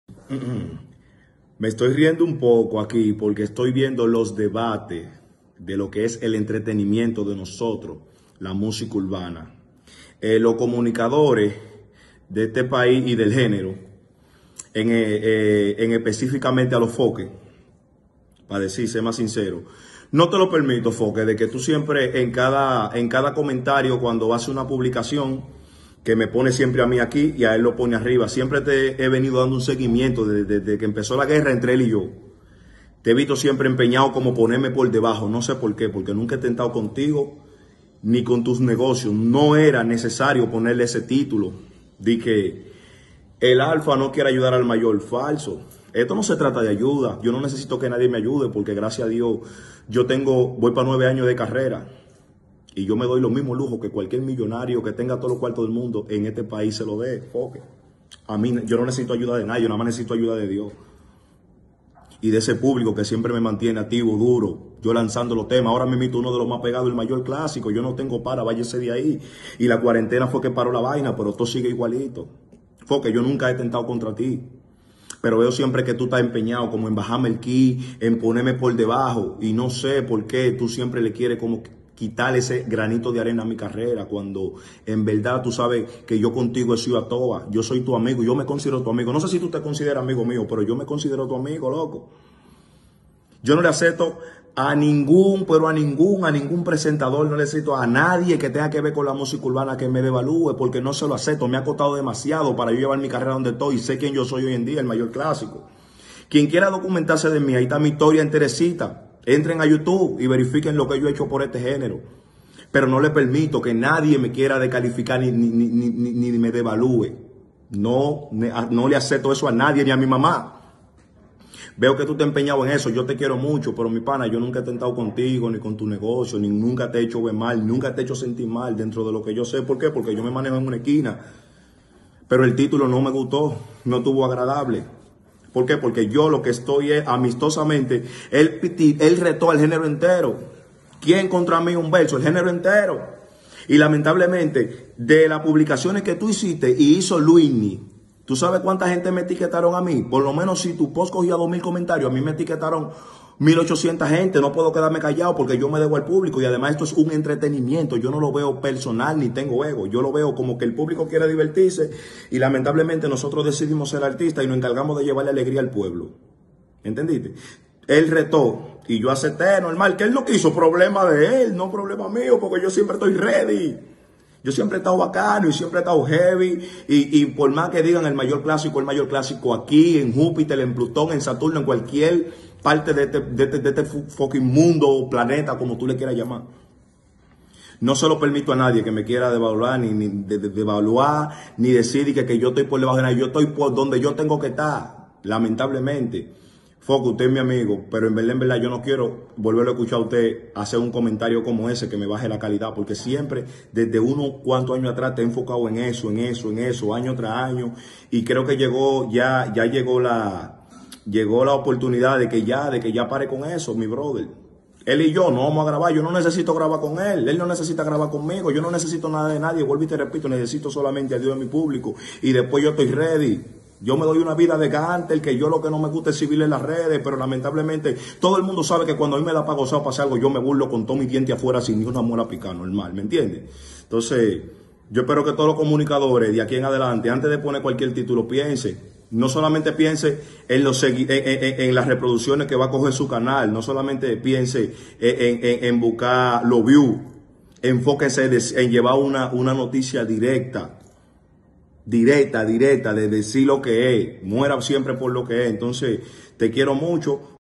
El Mayor Clásico en disputa con Alofoke por El Alfa
El desahogo del Mayor Clásico...